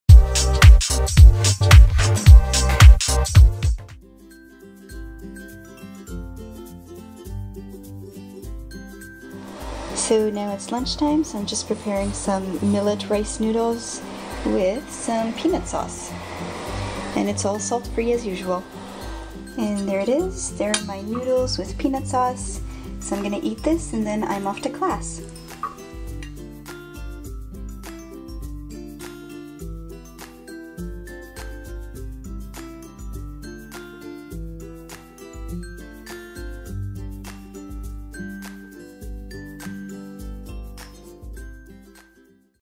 Day 14 - 30 Day Greens Challenge
My first class was great!  Went out with some friends afterwards, but stuck to my veggie-munching routine :)